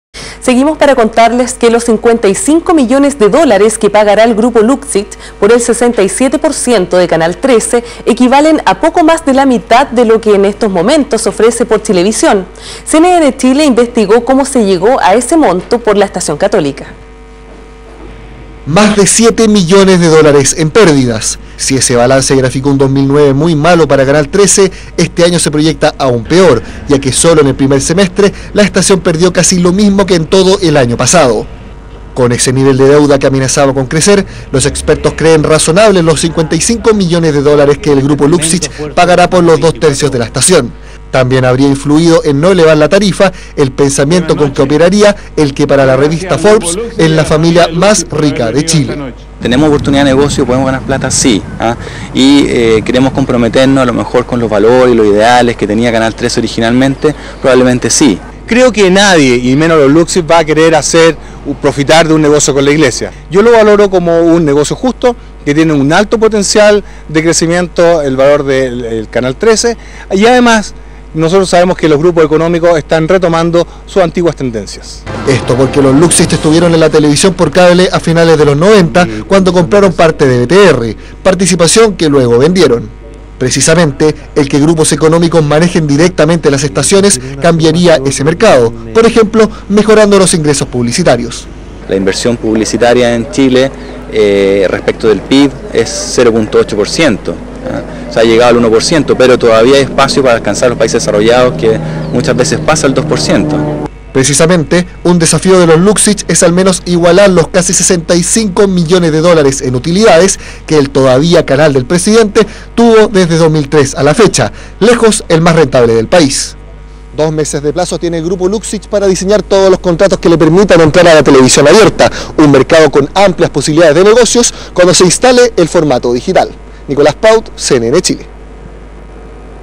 Grupo Luksic entra al negocio de la televisión
Crónica sobre la compra de Canal 13 y el ingreso de nuevos grupos económicos a los canales de televisión, lo que estaría generando un nuevo mercado. Fuente citada: Franco Parisi, decano de IEDE Escuela de Negocios de la U. Andrés Bello.